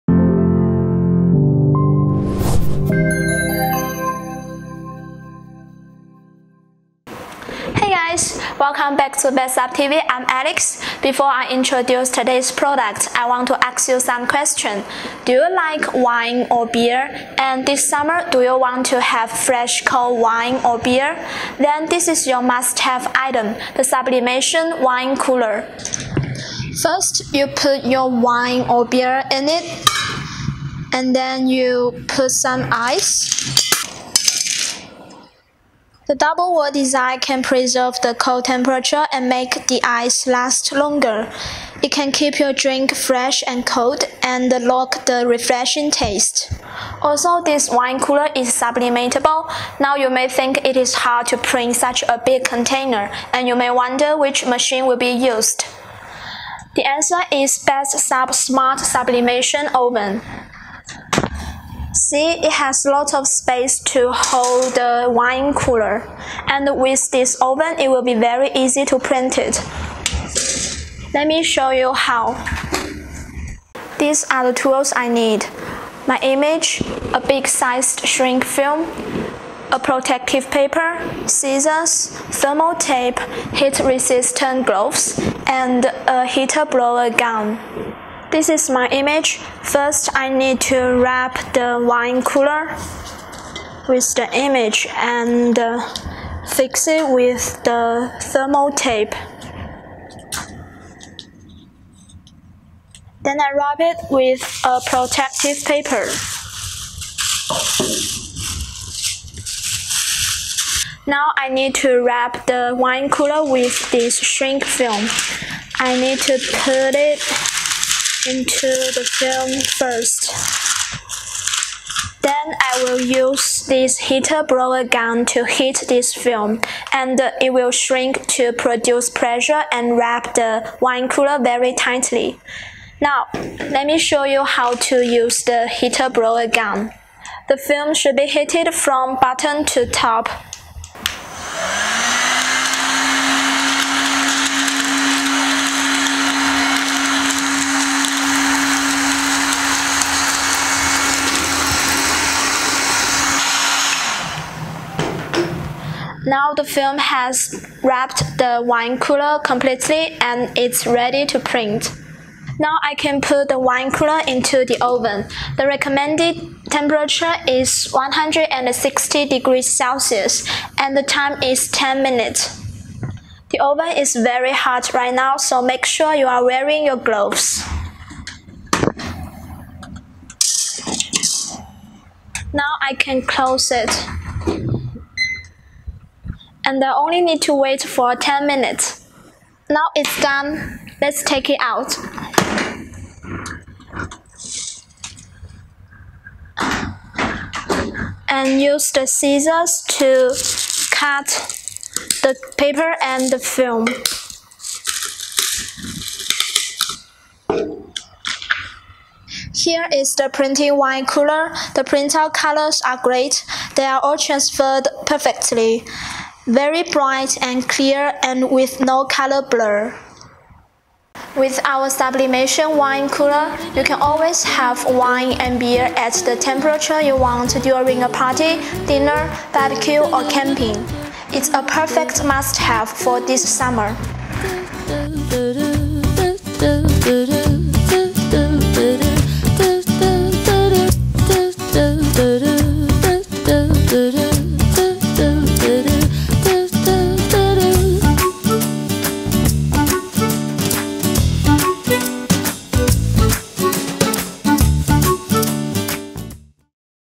Why Not Print a Sublimation Wine Cooler for This Summer?
Do you like wine or beer? This summer, do you want to have fresh cold wine or beer any time? Then you can’t miss BestSub Sublimation wine cooler. It can preserve the cold temperature and make the ice last longer, and keep your drink fresh and cold, and lock the refreshing taste. You may wonder how to print such a big container. In this video, we will show you how to print. It’s very easy to print the wine cooler with our smart sublimation oven. Check out the video.